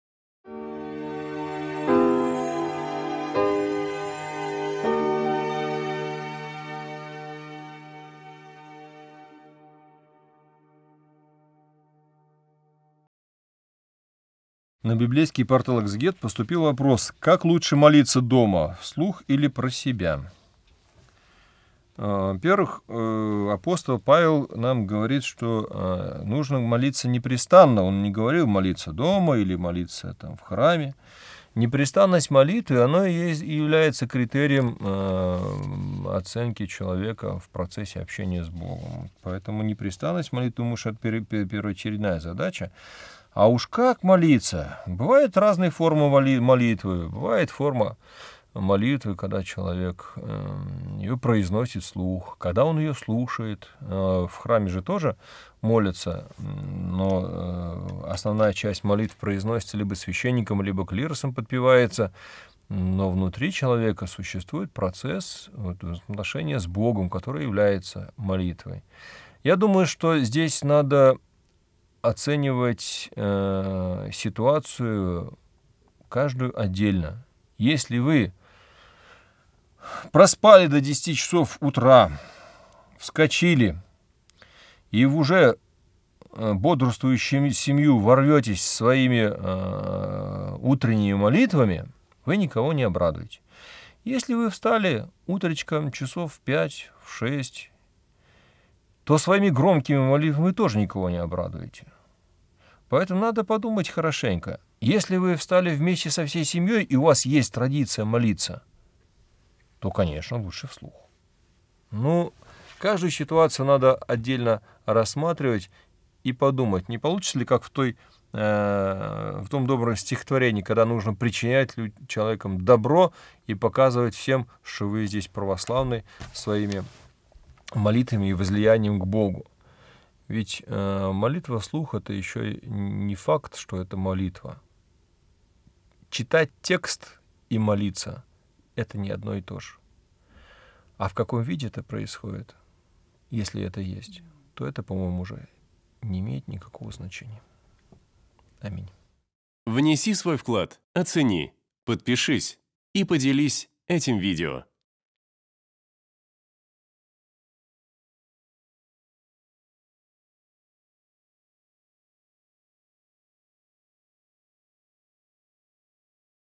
Как лучше молиться дома : вслух или про себя ? Протоиерей Александр Тылькевич
В сегодняшнем видео вместе с протоиереем Александром Тылькевичем отвечаем на вопрос: "Как лучше молиться дома :  вслух или про себя ?" Касаемся следующих тем:

🔸 Непрестанная молитва.
🔸 Молитва в храме.
🔸 Дисциплина христианина.
🔸 Верующая семья.

📜 Читать 1-ое послание к Фессалоникийцам ап. Павла 5 глава 17 стих (с толкованиями)